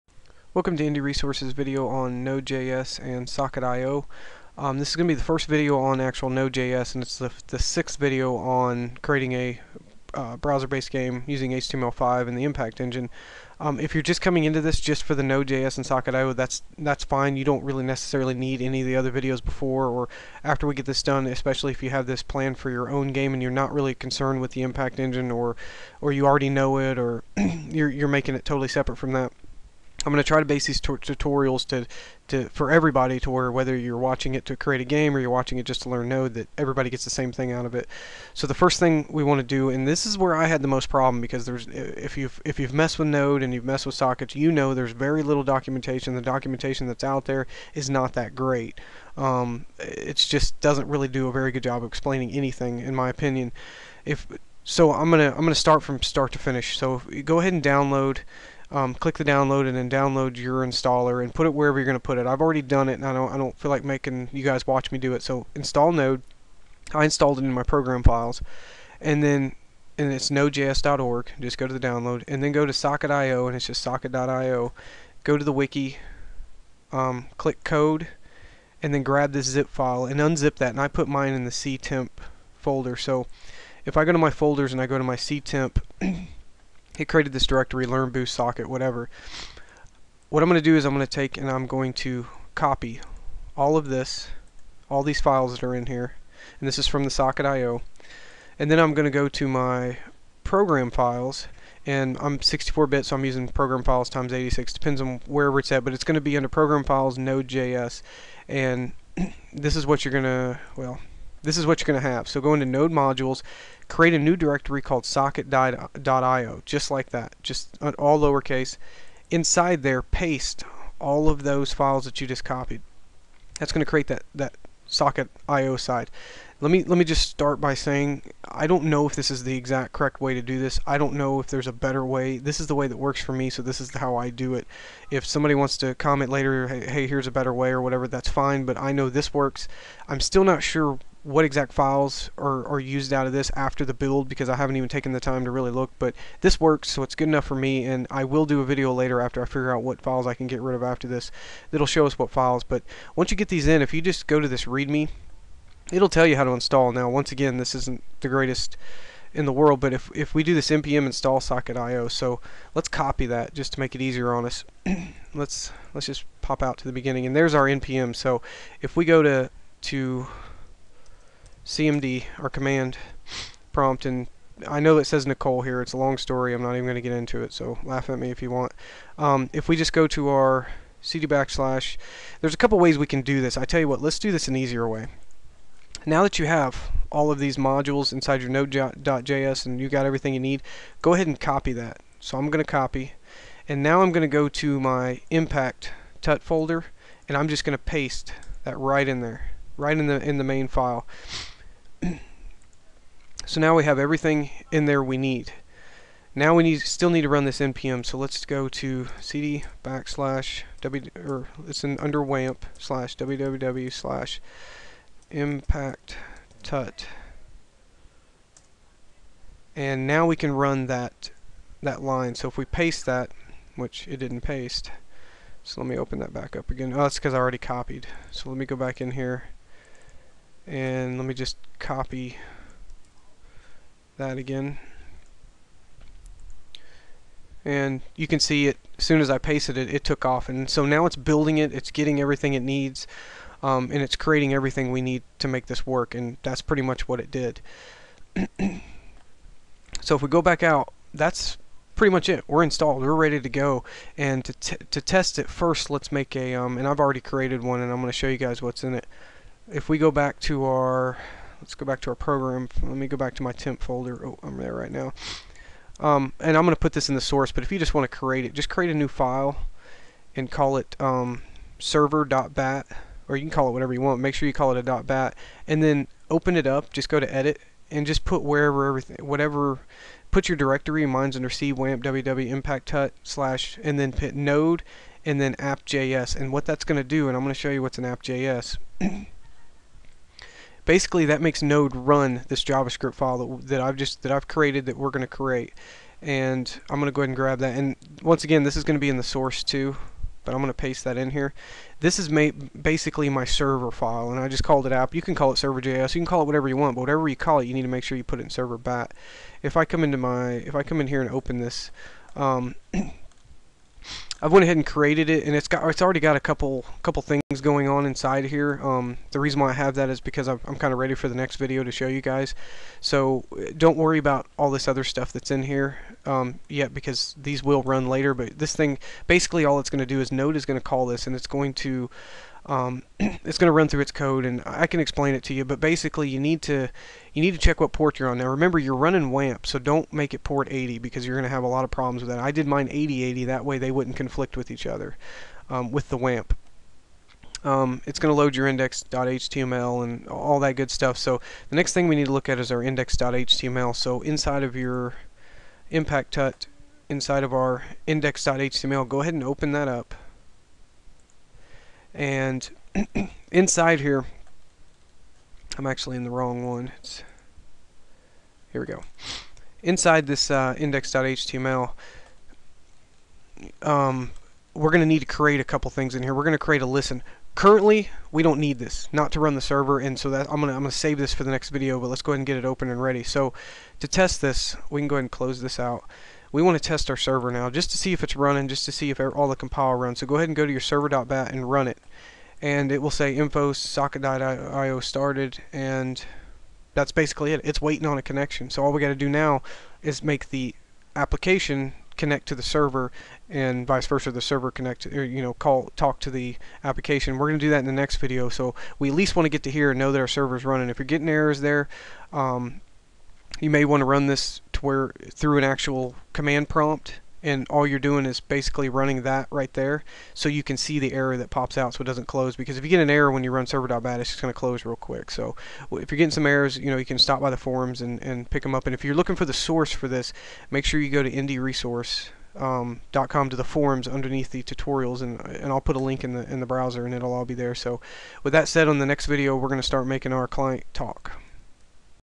Node.js and Socket.io part 1 Building a HTML5 game part 6
Part 6 in our HTML5 series but part 1 of the node  and socket series. Can be watched on its own.  Watch this and the next video before downloading source. You can grab all this straight from the source if you want.